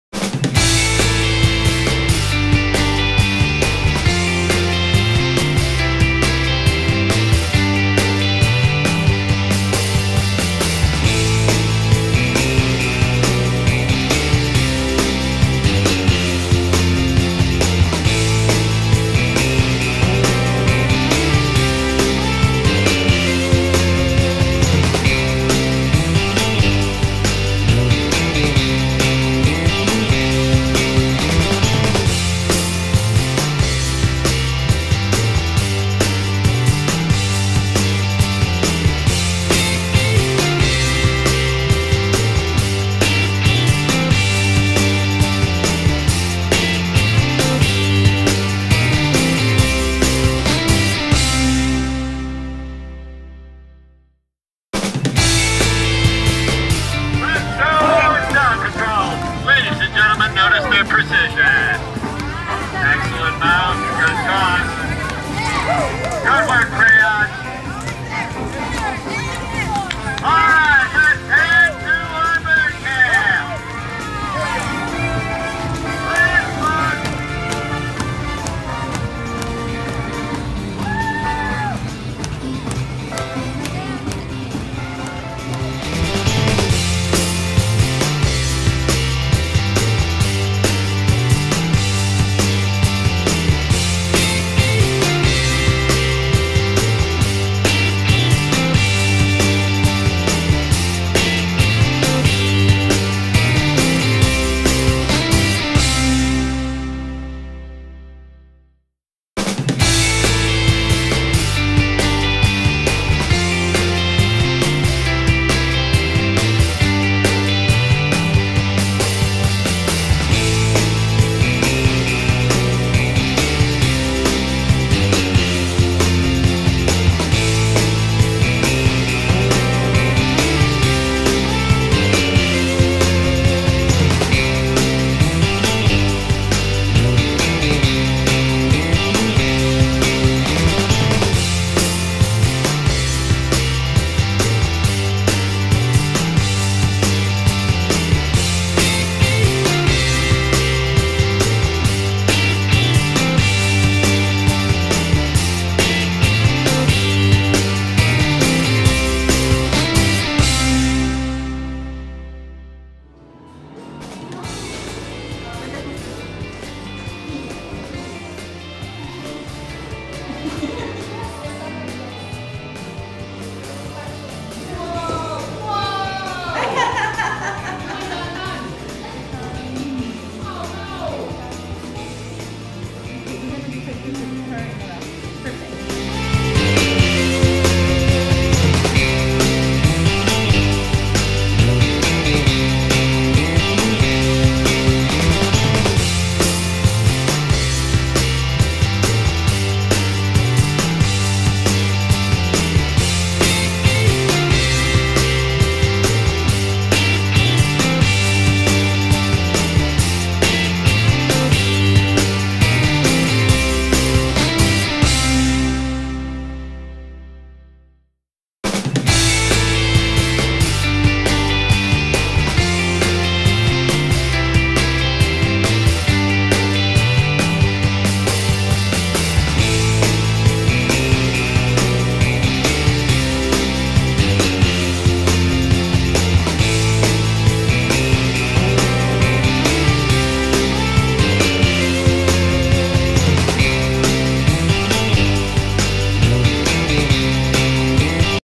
Maryellen’s Disney photos
Maryellen had so much fun at Disney’s Hollywood Studios. Here is her photo album from her visit.